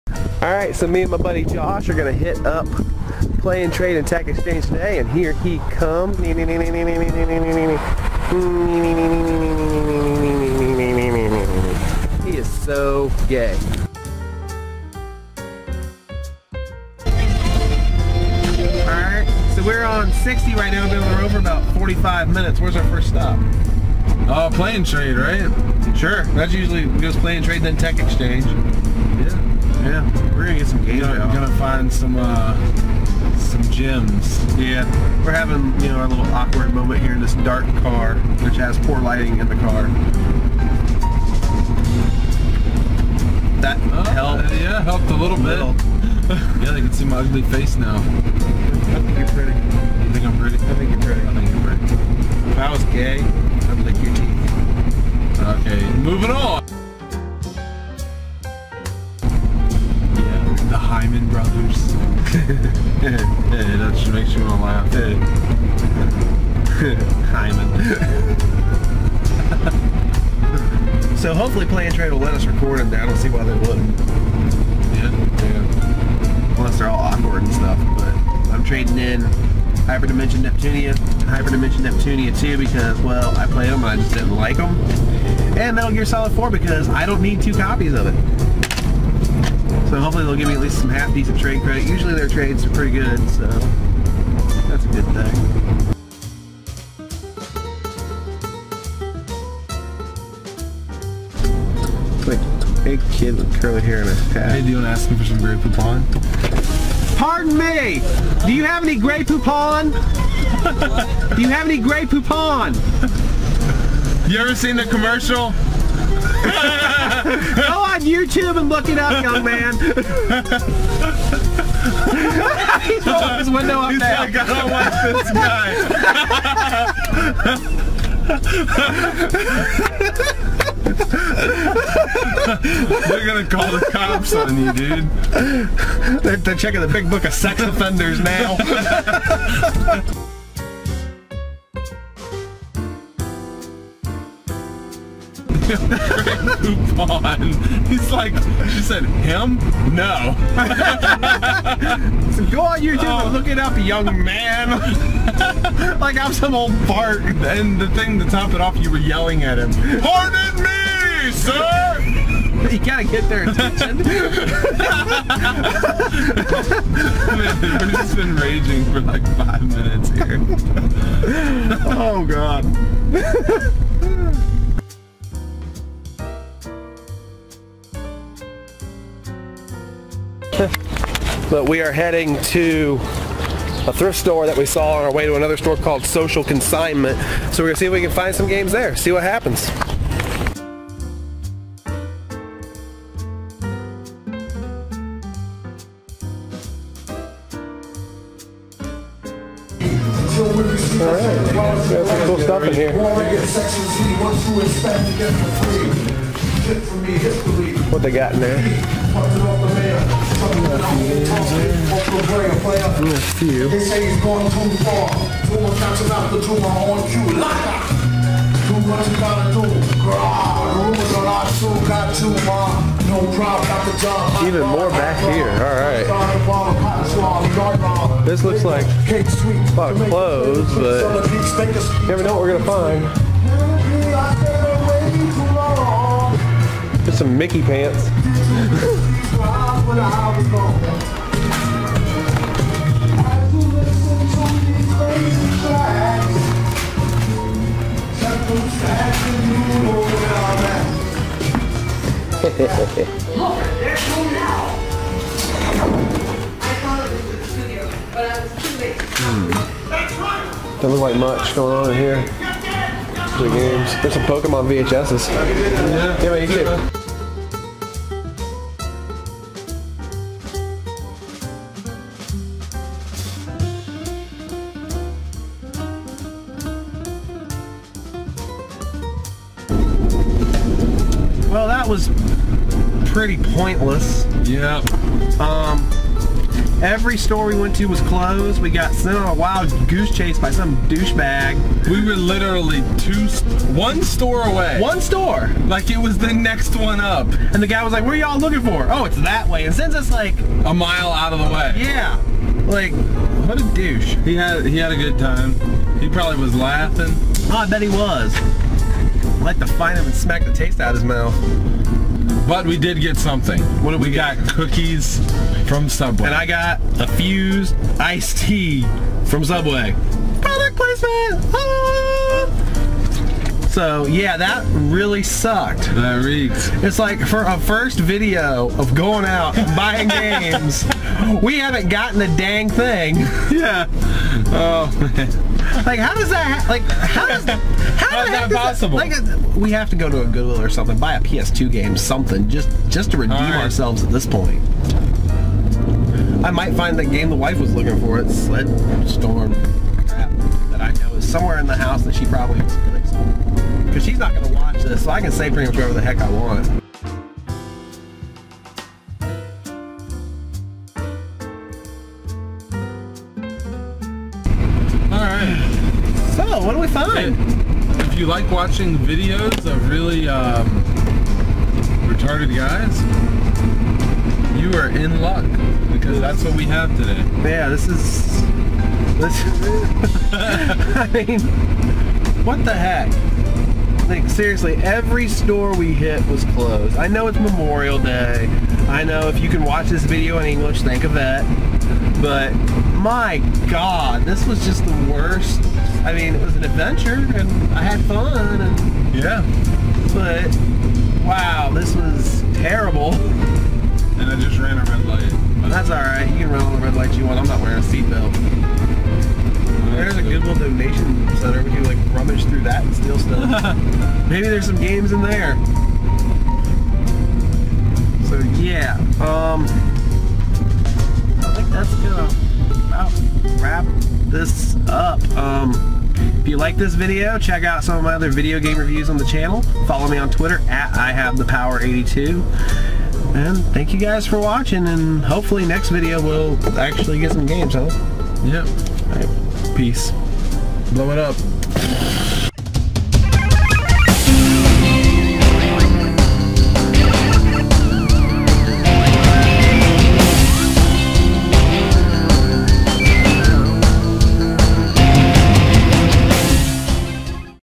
The Multiplayers: Ep 01 The Search for nothing
Hey guys!
Here is the first episode in a new series chronicling myself Genesis Moss and my good buddy Josh Davis in our search for video games across Virginia and hopefully across the east coast one day! In this first rocky episode we learned a lot about what we need for filming something like this so please stick with us and keep watching!